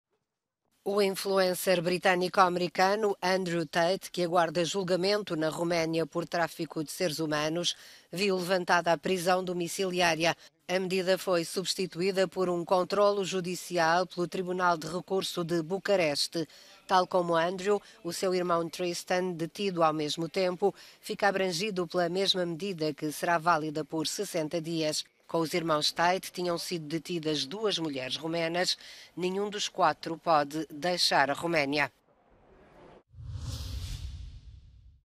Roménia: Irmãos Tate deixam prisão domiciliária
Andrew e Tristan Tate viram a medida de coação aliviada pelo tribunal de Bucareste, mas não podem sair da Roménia.